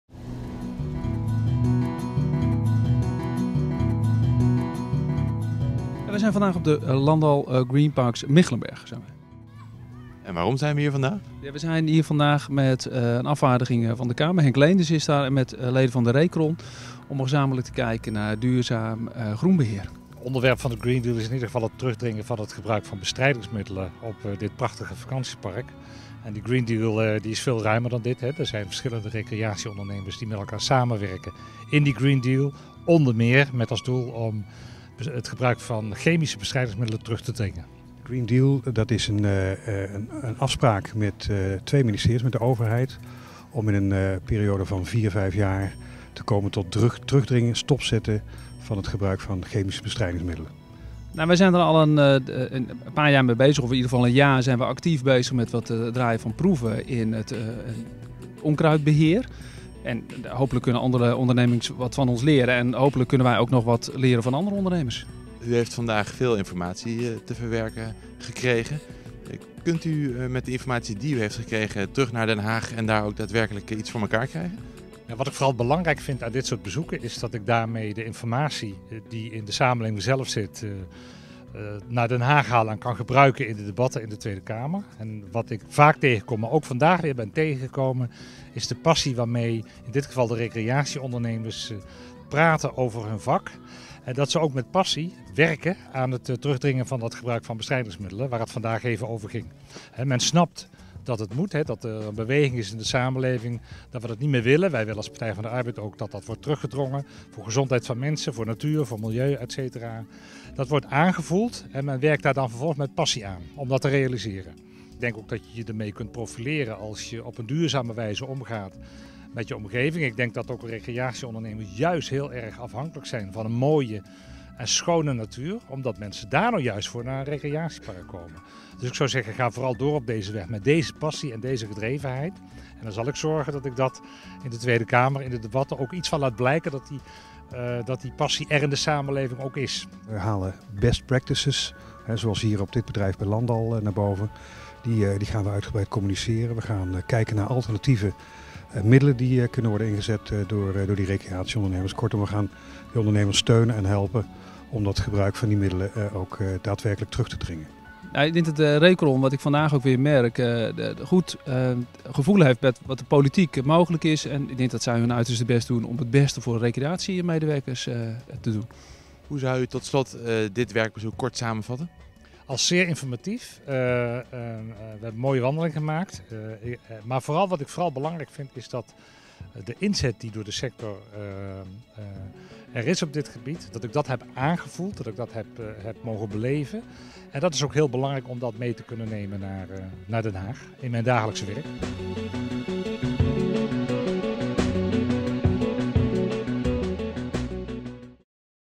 Werkbezoek PvdA aan Landal Miggelenburg over duurzaam groenbeheer
Werkbezoek kamerlid Henk Leenders (PvdA) aan Landal De Miggelenberg. Onderwerp Duurzaam Groenbeheer. Aanleiding was de Green Deal die gesloten is op donderdag 22 oktober met Ministeries van Infrastructuur en Milieu en Economische Zaken, RECRON, HISWA, Landal GreenParcs, Centerparcs en Roompot.